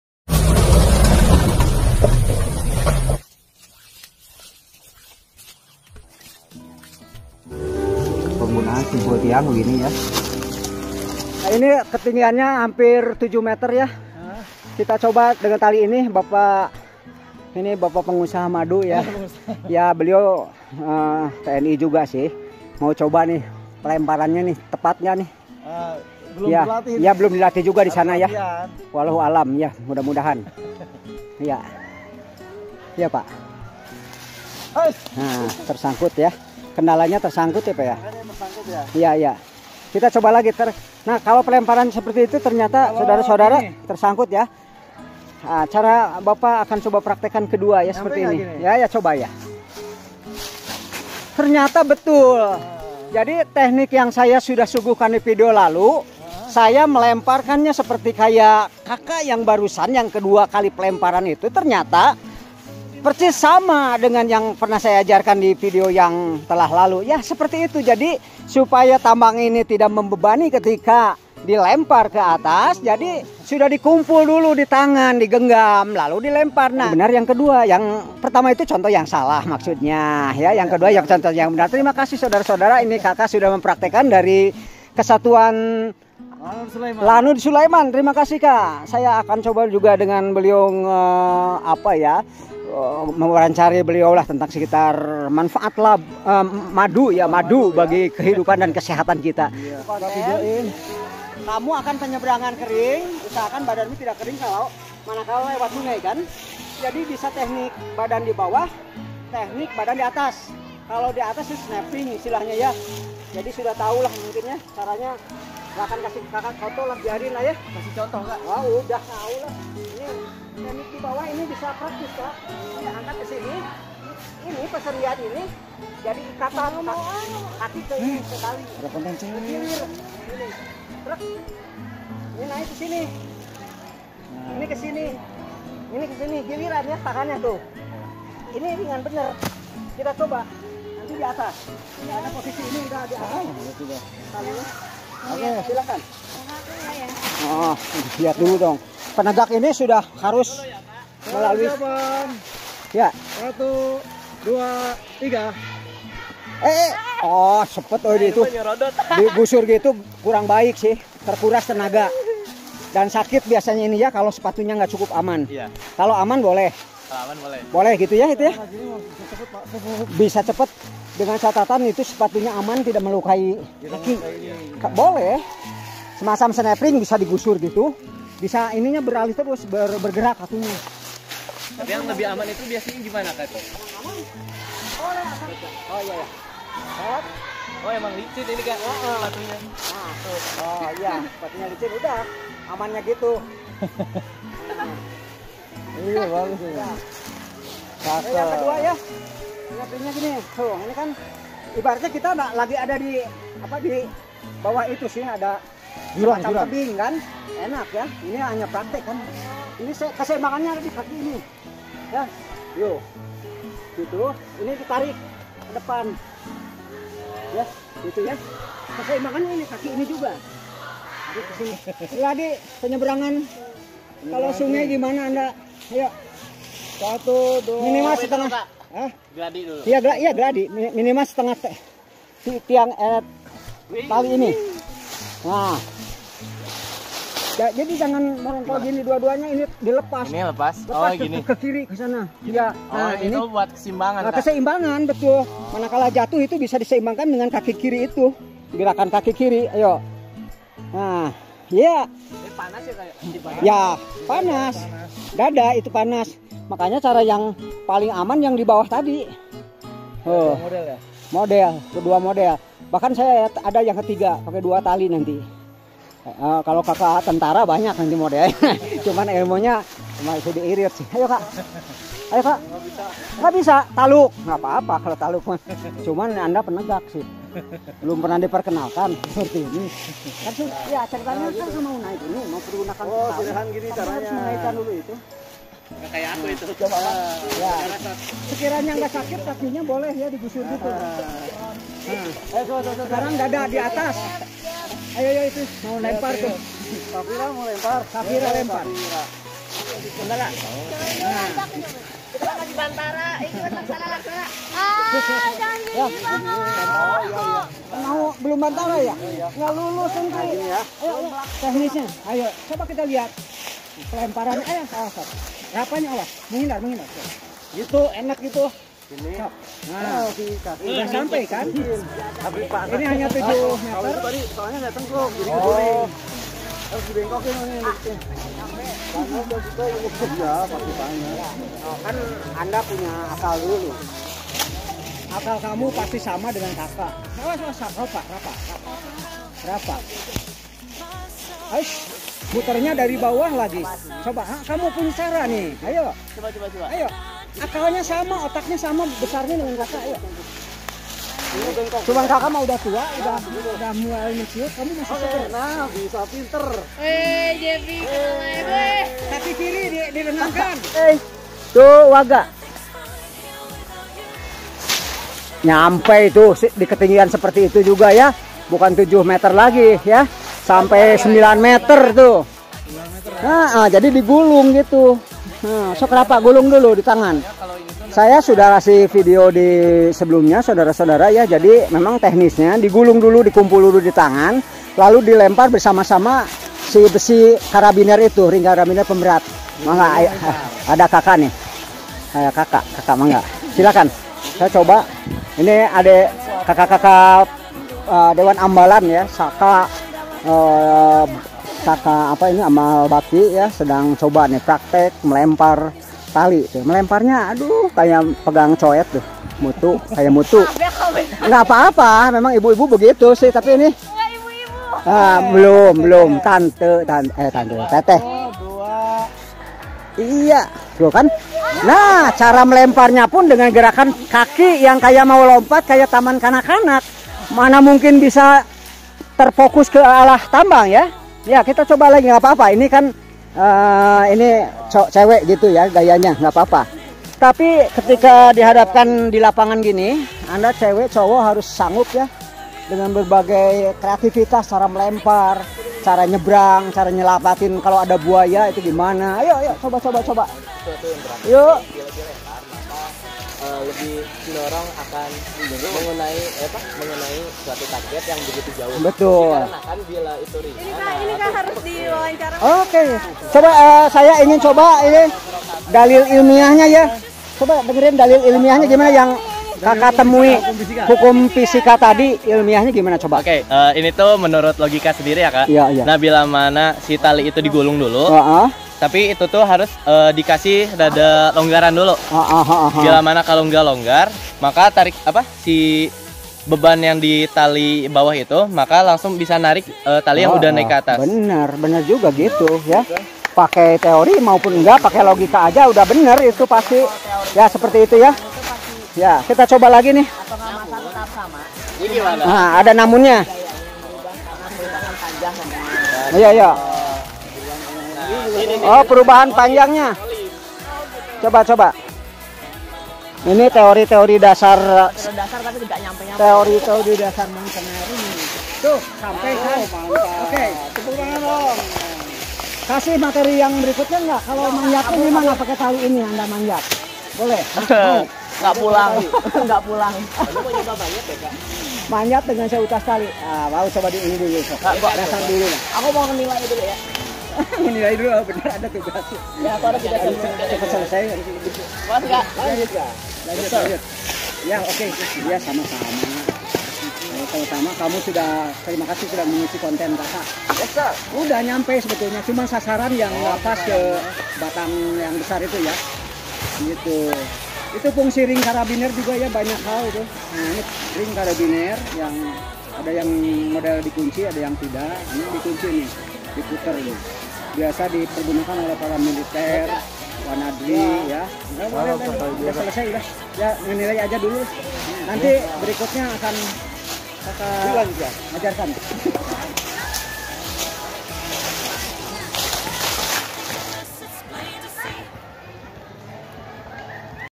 Teknik Melempar dan Menyeberang Tali Ala Pramuka dan Militer.
Antara Kesatuan Militer dan Pramuka mwmiliki kesamaan dalam hal melempar dan menyeberang dengan seutas tali. Hal ini dibuktikan dalam praktek video Jejak Pemandu ini.